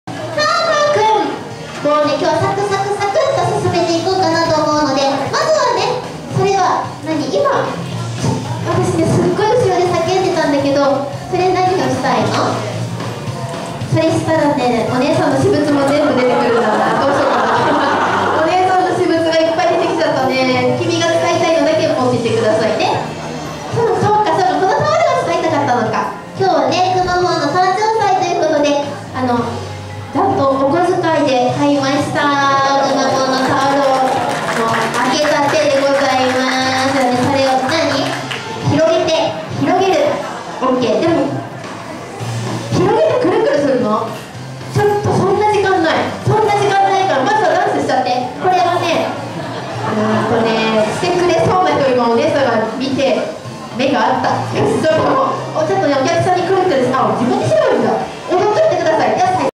190310_かぁくん[01]タオルほしい
「くまモン誕生祭2019」で「くまモンスクエア」に出没した、みやざき犬の『かぁくん』。タオルを取り出したくて鞄の中身を舞台にぶちまける。みやざき犬